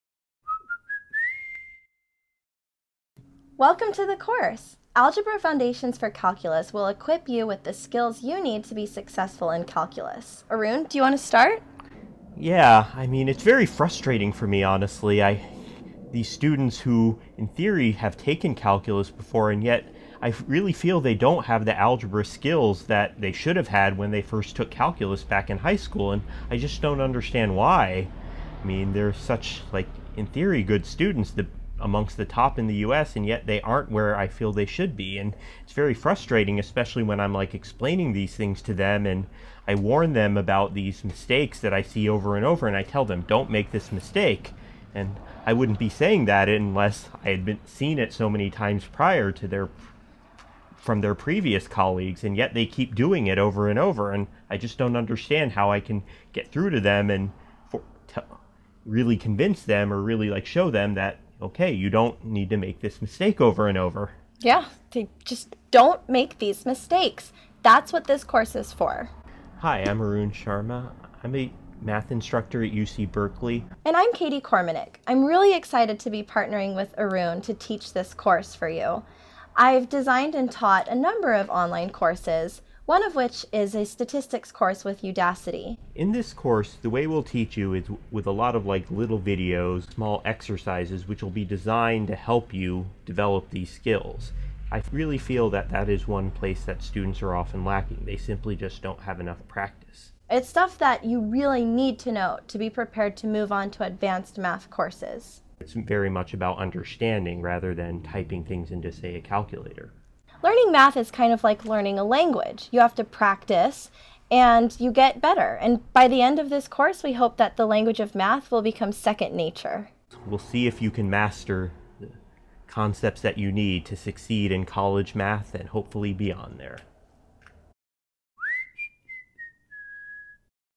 TURN THE WHEEL: Algebra Foundations for Calculus promo
Hear about our first of many online course offerings, Algebra Foundations for Calculus featuring Arun Sharma, PhD, Calculus professor at UC Berkeley. This course teaches what you need to know to be successful in Calculus and beyond. Visit turnthewheel.org to sign up!